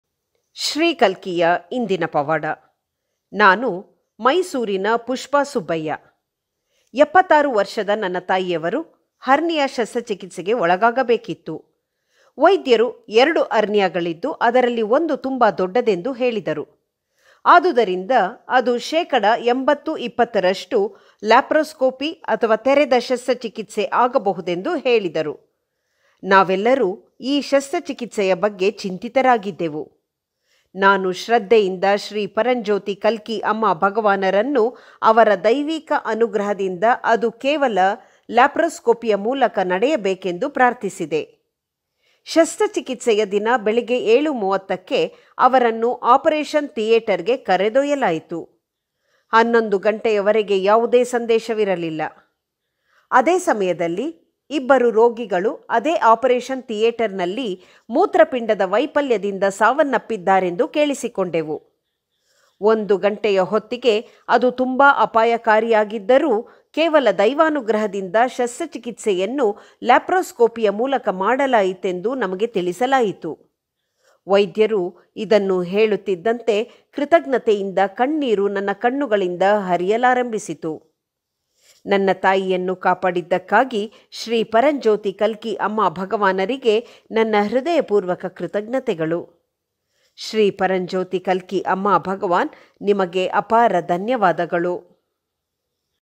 21/03. ದೈವಾನುಗ್ರಹದಿಂದ ಮಾತ್ರವೇ ಶಸ್ತ್ರಚಿಕಿತ್ಸೆಯ ಕಾರ್ಯವನ್ನು ಲಾಪೆÇ್ರೀಸ್ಕೋಪಿ ಮೂಲಕ ನಡೆಸಲಾಯಿತು.
ನಾನು ಶ್ರದ್ದೆಯಿಂದ ಶ್ರೀ ಪರಂಜ್ಯೋತಿ ಕಲ್ಕಿ ಅಮ್ಮಭಗವಾನರನ್ನು ಅವರ ದೈವಿಕ ಅನುಗ್ರಹದಿಂದ ಅದು ಕೇವಲ ಲಾಪ್ರೋಸ್ಕೋಪಿಯ ಮೂಲಕ ನಡೆಯಬೇಕೆಂದು ಪ್ರಾರ್ಥಿಸಿದೆ.
ನನ್ನ ತಾಯಿಯನ್ನು ಕಾಪಾಡಿದ್ದಕ್ಕಾಗಿ, ಶ್ರೀ ಪರಂಜ್ಯೋತಿ ಕಲ್ಕಿ ಅಮ್ಮಭಗವಾನರಿಗೆ ನನ್ನ ಹೃದಯಪೂರ್ವಕ ಕೃತಜ್ಞತೆಗಳು. ಶ್ರೀ ಪರಂಜ್ಯೋತಿ ಕಲ್ಕಿ ಅಮ್ಮಭಗವಾನ್! ನಿಮಗೆ ಅಪಾರ ಧನ್ಯವಾದಗಳು.